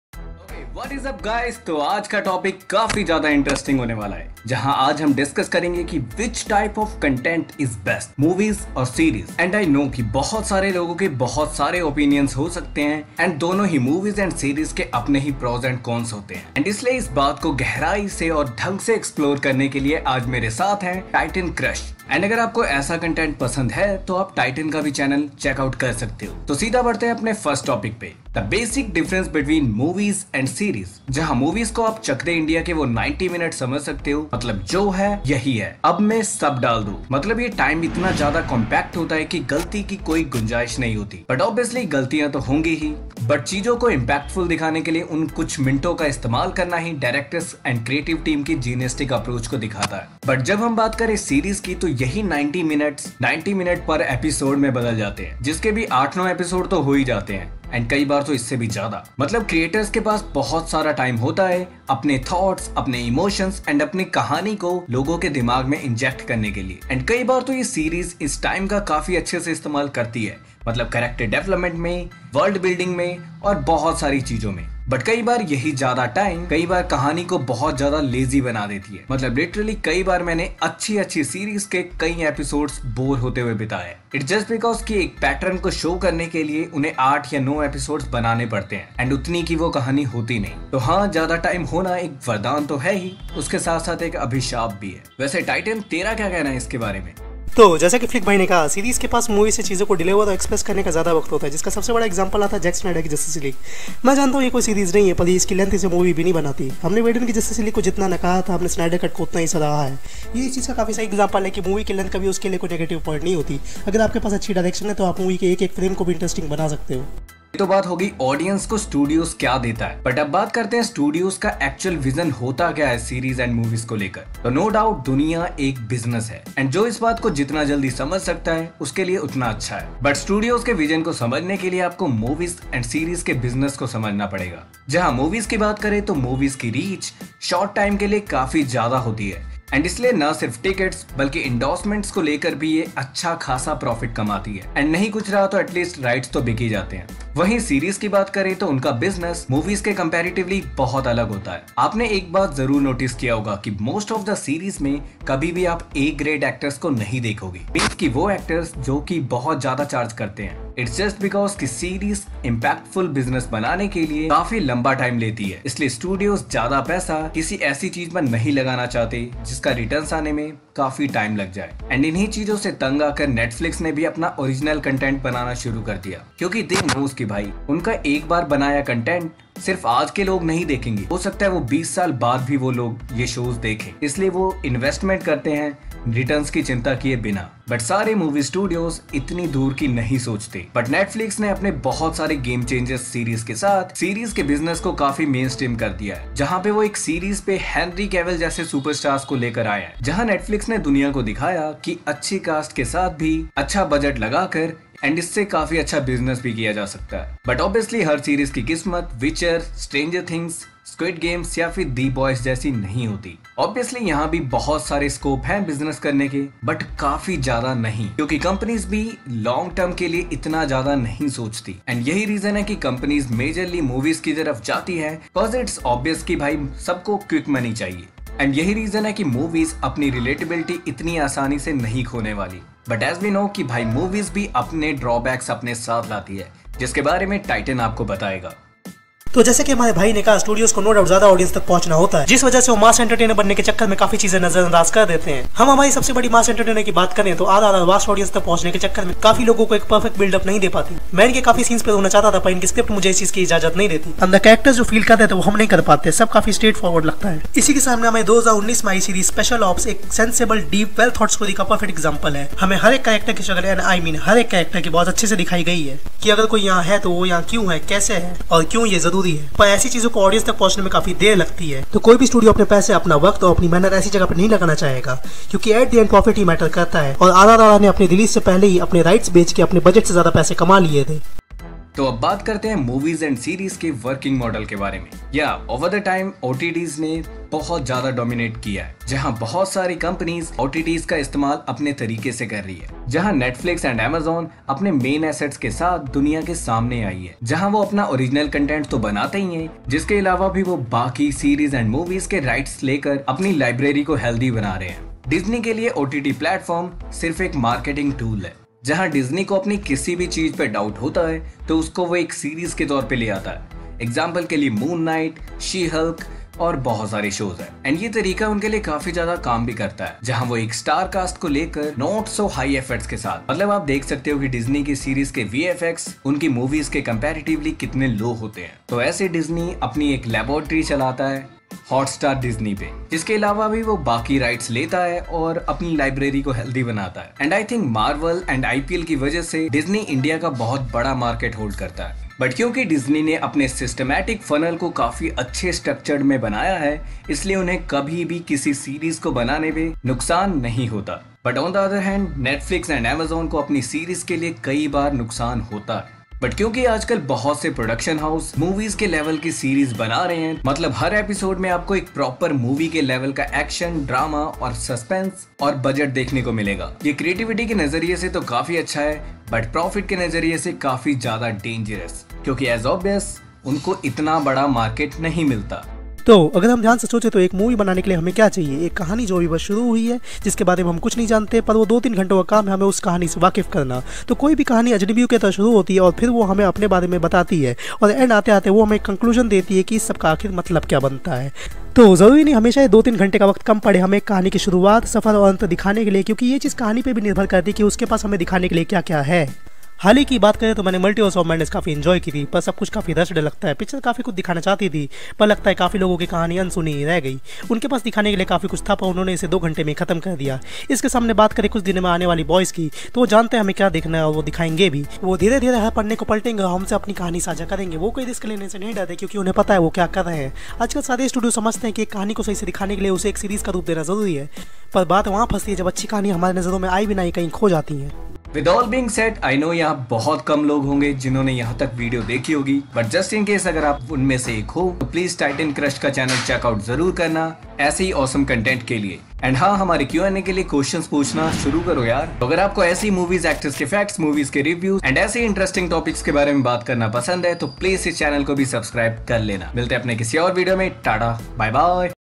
Movies or Webseries Which is Better
We discussed about how movies and series makes money.